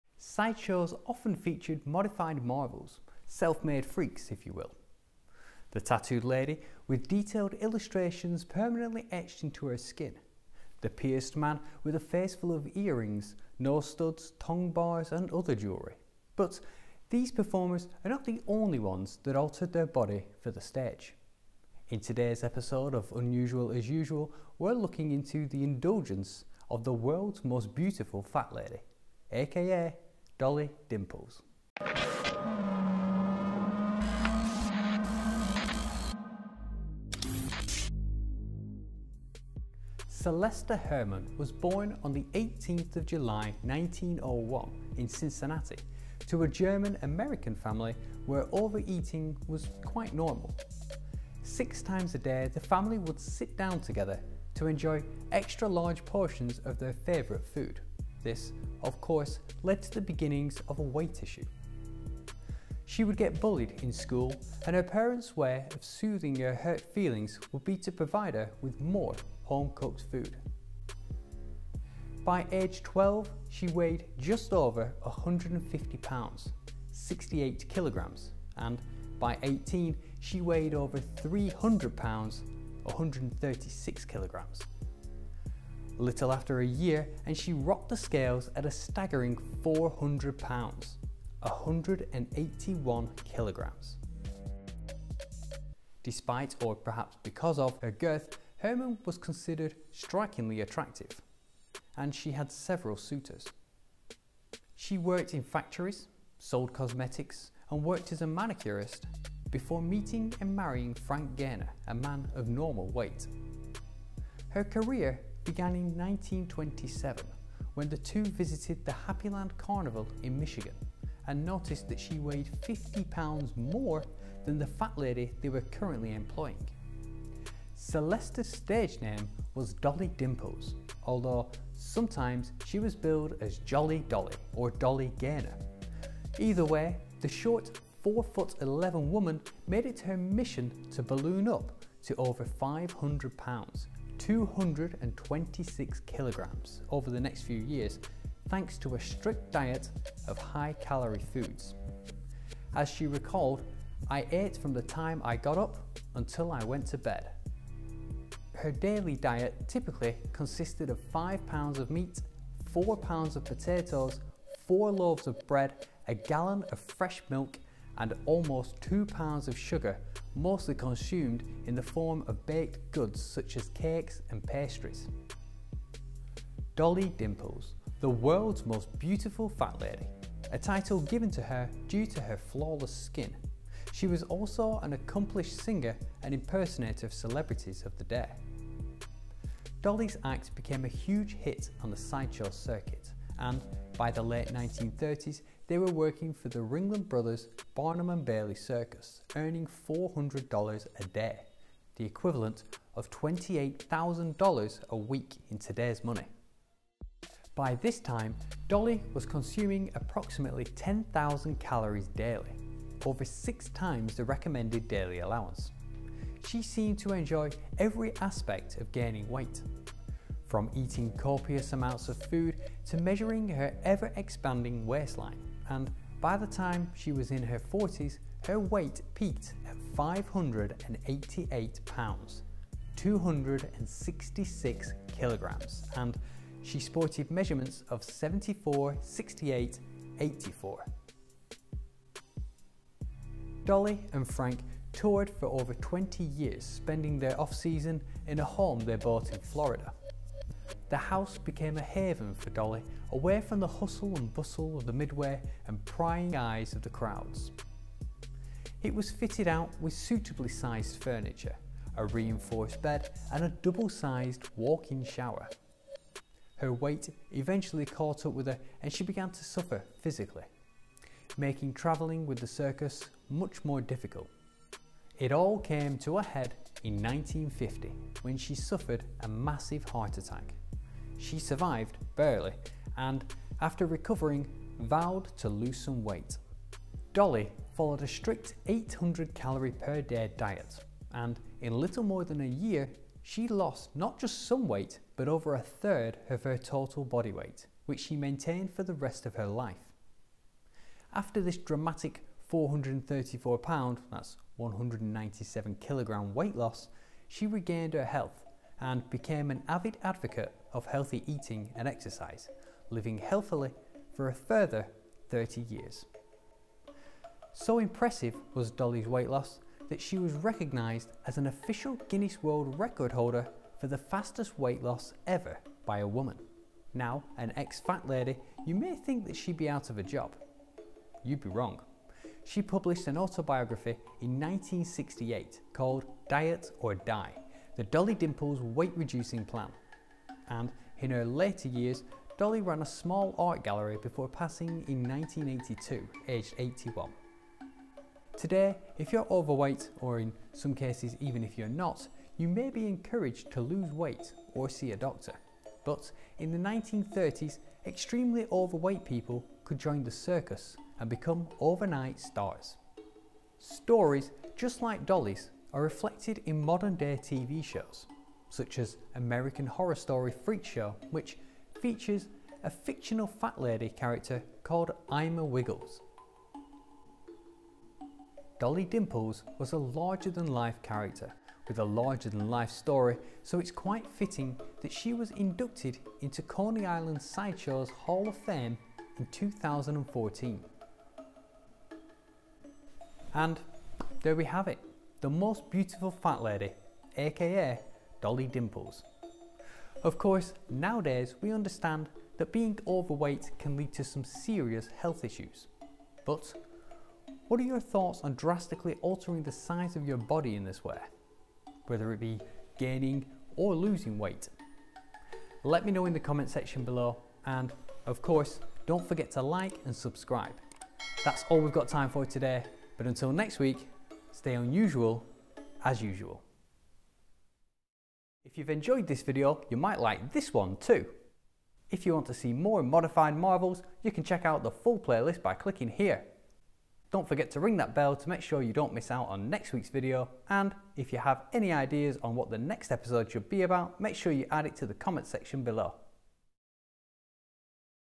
The Indulgence of Dolly Dimples
Celesta Herrmann was born on 18th July 1901 in Cincinnati to a German-American family where over eating was normal. Six times a day, the family would sit down together to enjoy extra large portions of their favourite food, this of course lead to the beginnings of a weight issue.

She would get bullied in school, and her parents' way of soothing her hurt feelings was to provide her with more home-cooked food. By age 12 she weighed just over 150 lbs (68 kg) and by 18 she weighed over 300 lbs (136 kg), A little after a year and she rocked the scales at a staggering 400 lbs (181 kg).

In 1950 she survived a heart attack which inspired her to published an autobiography called "Diet or Die; The Dolly Dimples Weight Reducing Plan”.

Following her strict 800 calorie per day diet helped her lose over a third of her total weight and she was recognised as an official Guinness World Record holder for "the fastest weight loss ever by a woman”. She died in 1982 aged 81.

Fact Analysis: Although careful research is implemented to assure accurate and correct information, sometimes it can be difficult to separate fact from fiction (or ‘humbug', as P.T. Barnum would say).